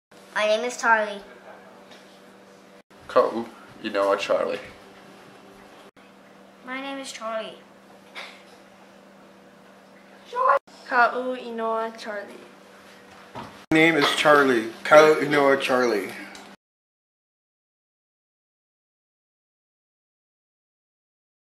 JeSuisCharlie KauInoaCharlie
January 8, 2015 In solidarity with Charlie Hebdo and all the victims of the ParisShooting

Twitter Account(s): @OccHiloMedia - please follow and mention us on Twitter!